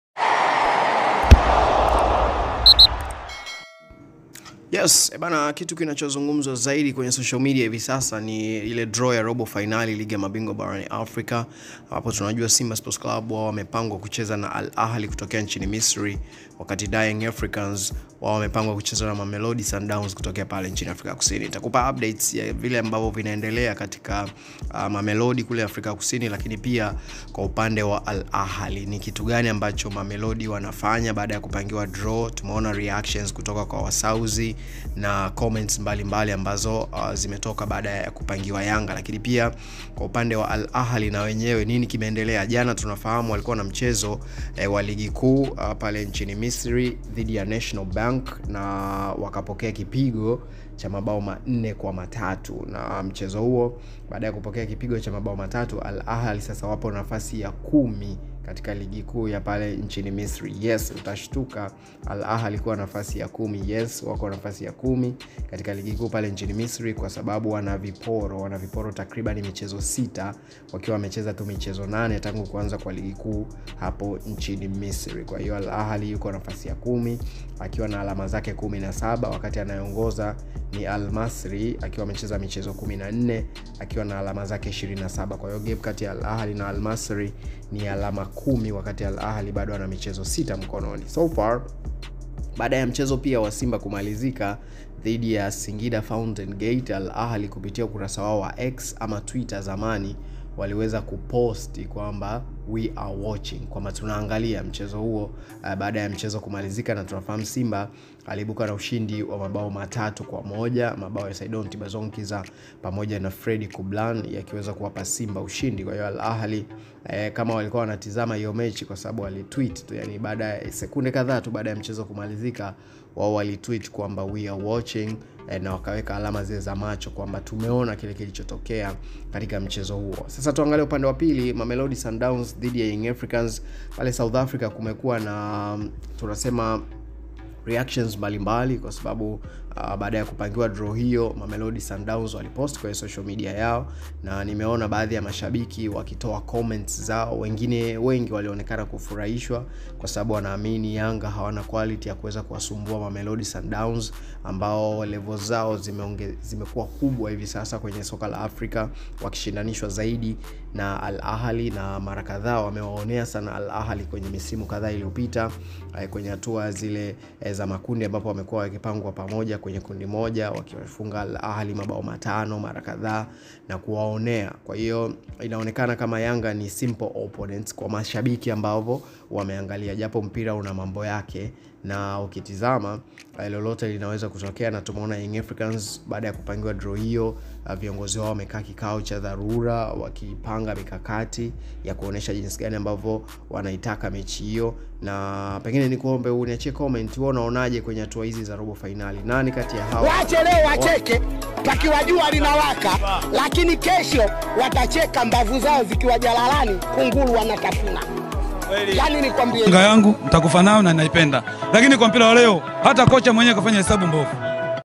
Umeskia Hiyo? Baada tu ya Kupangiwa Simba Ahly waibuka chamaz mech ya Singida,Mamelod wachekelea ku.
The reactions after simbasc drawn to alahly while yanga drawn against masandawana in totalenergiescafcl quarter finals